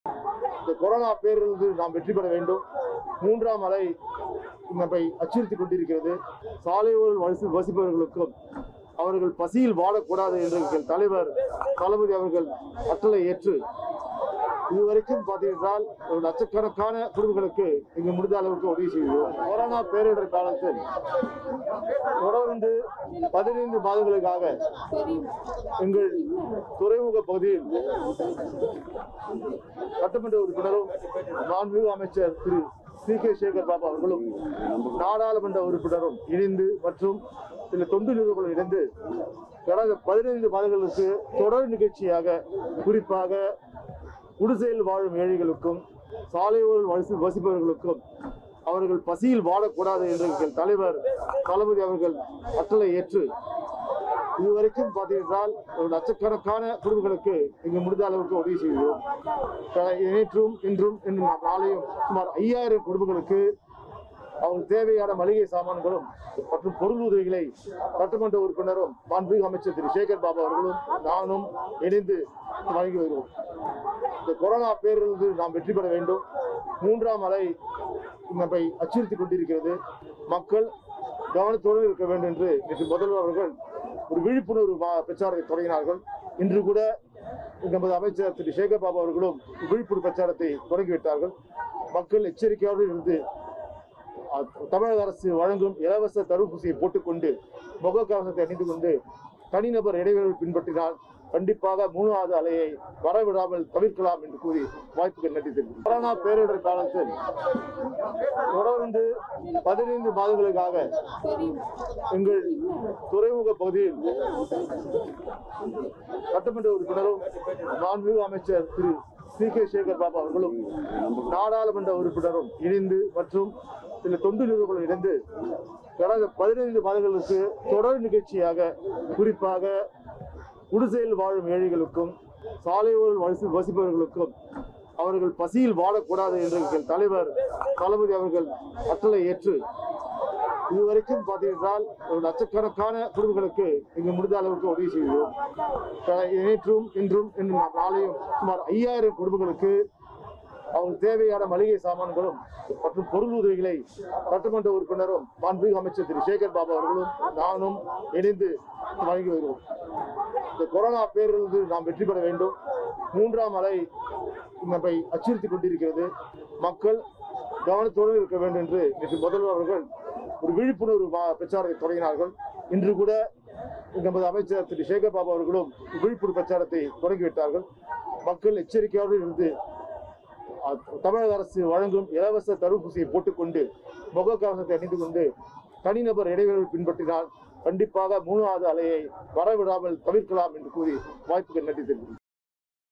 Dayanidhi Maran Latest Speech | கொரோனா 3'ஆம் அலையை நாம் வென்றாக வேண்டும் !
dayanidhi maran , dayanidhi maran latest speech , dayanidhi maran latest news , dayanidhi maran speech , dmk dayanidhi maran , covid 19